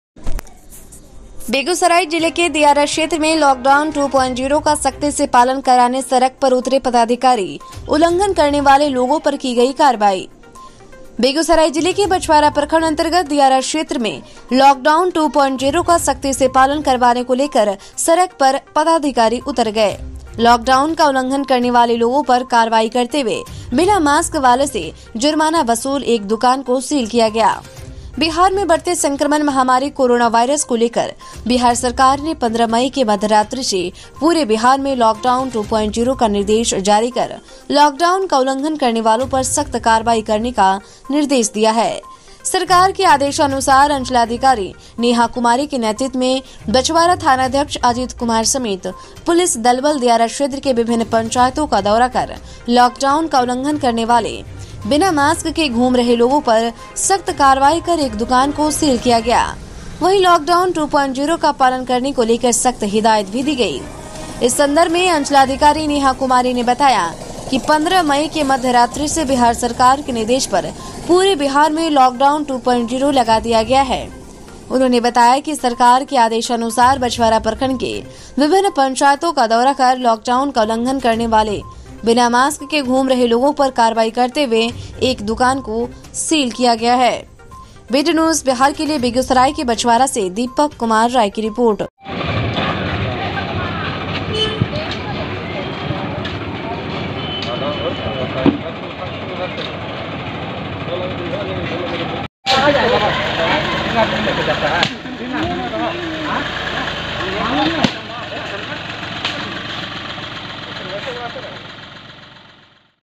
बेगूसराय:-दियारा क्षेत्र में लॉकडाउन 2.0 का सख्ती से पालन कराने सड़क पर उतरे पदाधिकारी,
बेगूसराय:-दियारा क्षेत्र में लॉकडाउन 2.0 का सख्ती से पालन कराने सड़क पर उतरे पदाधिकारी, उलंघन करने वालों पर की गई करवाई।BTNewsBihar